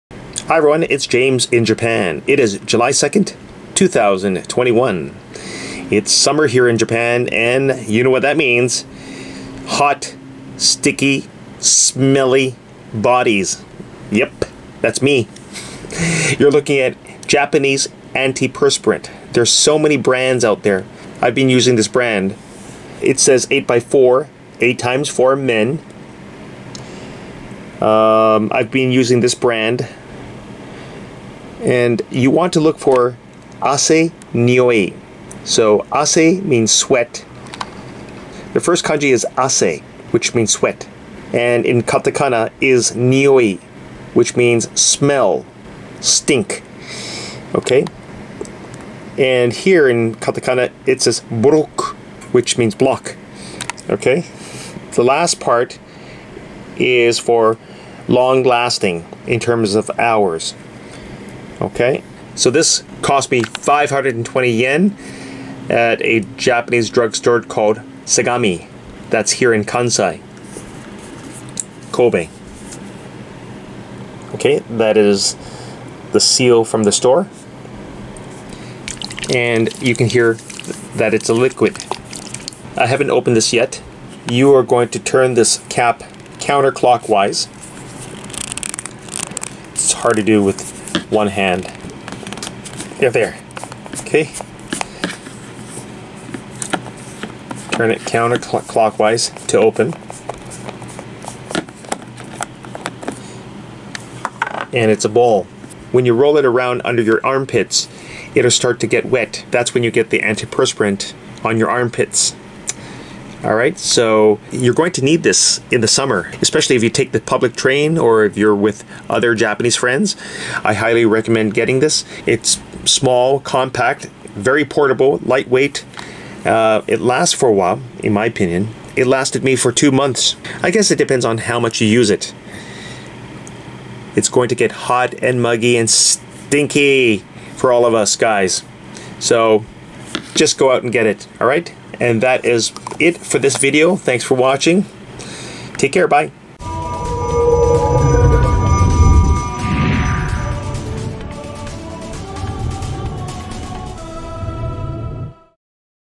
GUYS SMELL GOOD DURING THE SUMMER -Japanese Antiperspirant/Deodorant for Men-SMALL,COMPACT, PORTABLE
8x4 Men Roll On Fresh Soap 60ml Men Antiperspirant Deodorant
Blocks sweat and odors for a long time. One coat with big ball
Effects: Skin sweat odor, armpit smell, sweat proof

A roll-on deodorant that is effective for man's sweat and odor.

Highly adherent for man's smell.

Sweat and sterilization ingredients retain its effectiveness.The big ball makes it easy to apply. Uses a big ball that fits perfectly under the armpit for a cool and quick drying. It is comfortable to use even on busy mornings.